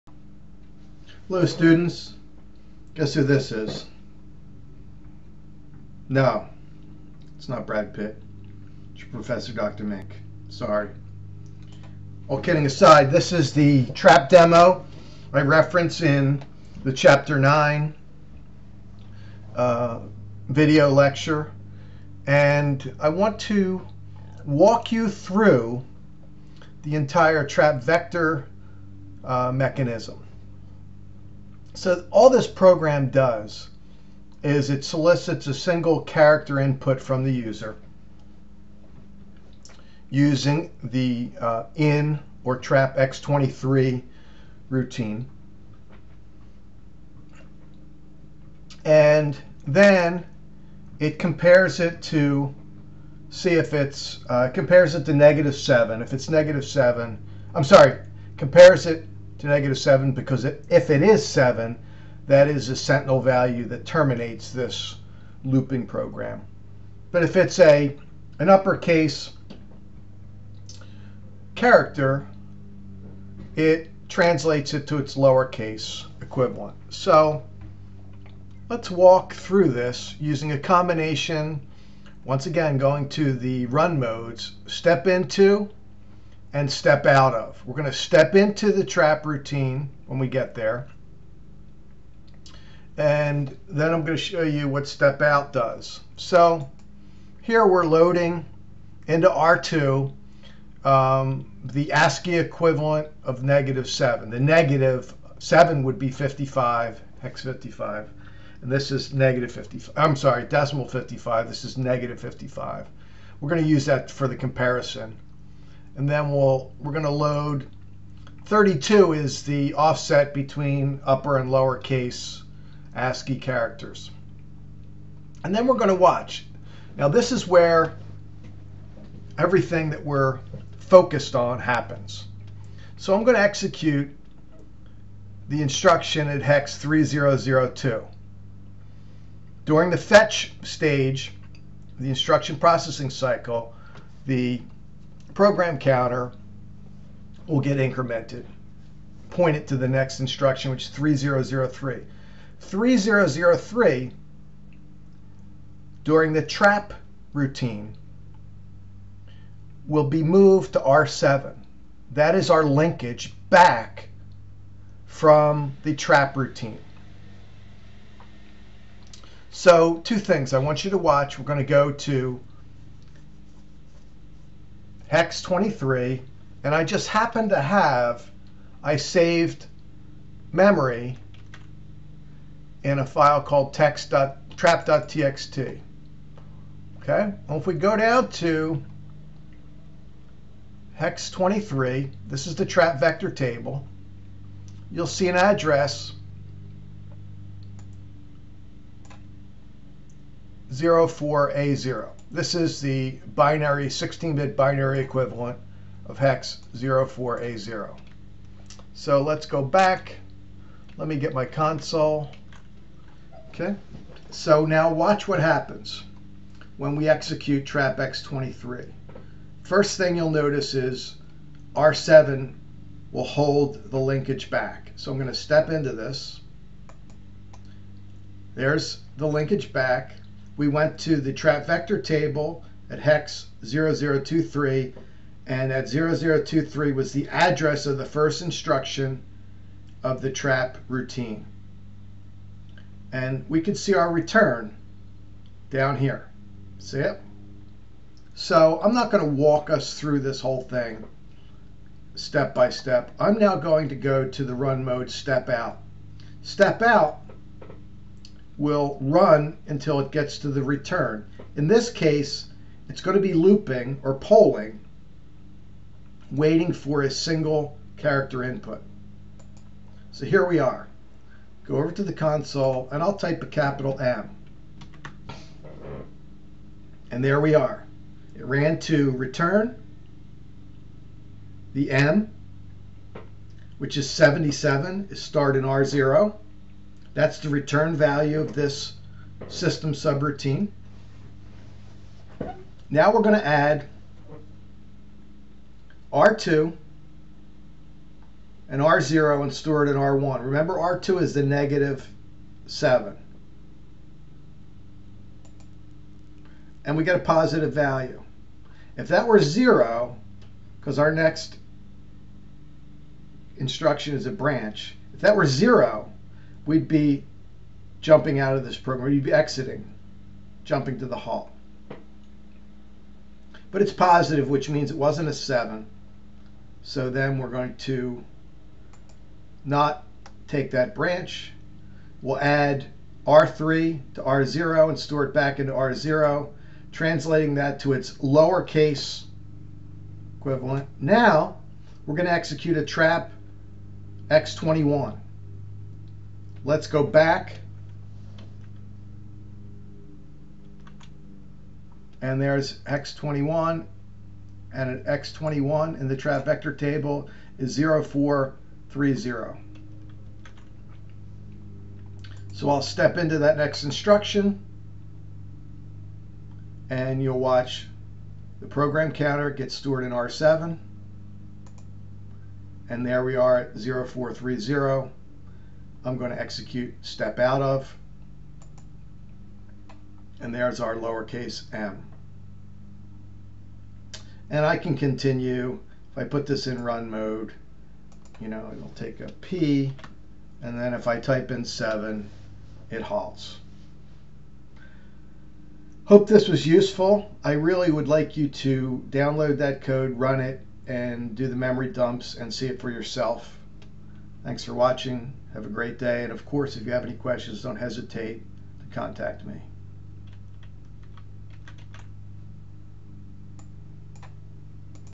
Trap Routine Demonstration
This video demonstrates the use of a LC-3 trap routine. It highlights use of the trap vector table and the return to the calling program.